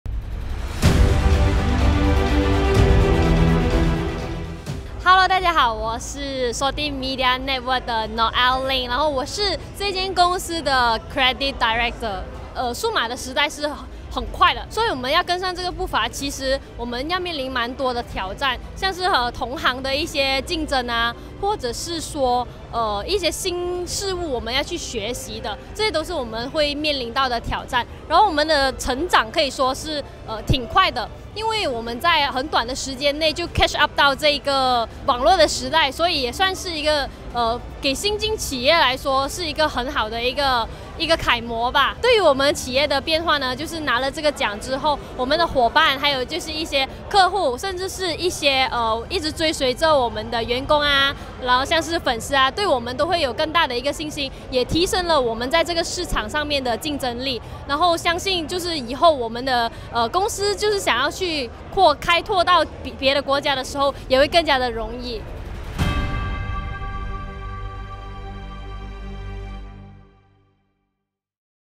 2023 Digital Transformation Awards Winner Interview - SALTY MEDIA NETWORK SDN BHD
Congratulations to SALTY MEDIA NETWORK SDN BHD, the winner of the Digital Transformation Awards 2023 !

To be part of the next Golden Bull Award,